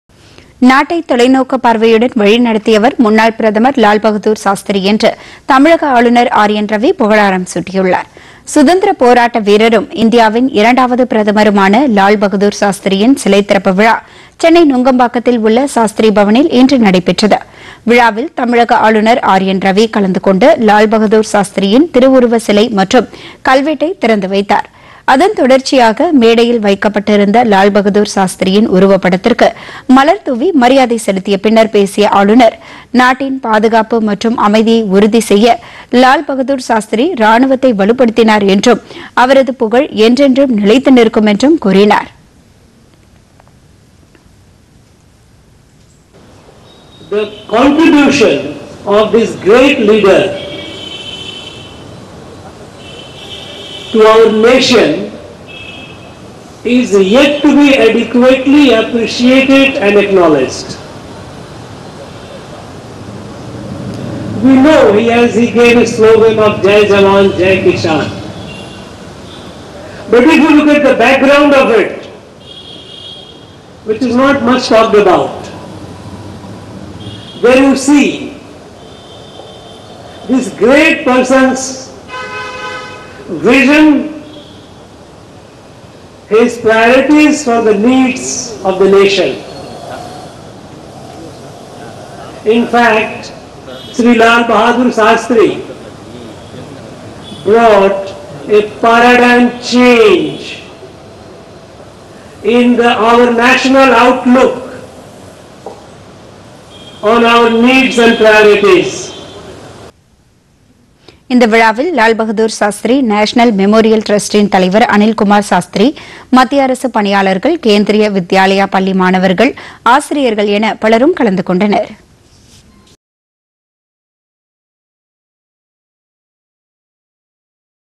லால்பகதூர் சாஸ்திரி சிலை திறப்புPodhigaiTamilNews
நாட்டை தொலைநோக்குப் பார்வையுடன் வழி நடத்தியவர் முன்னாள் பிரதமர் லால் பகதூர் சாஸ்திரி- சென்னை சிலை திறப்பு விழாவில் தமிழக ஆளுநர் ஆர் என் ரவி புகழாரம்.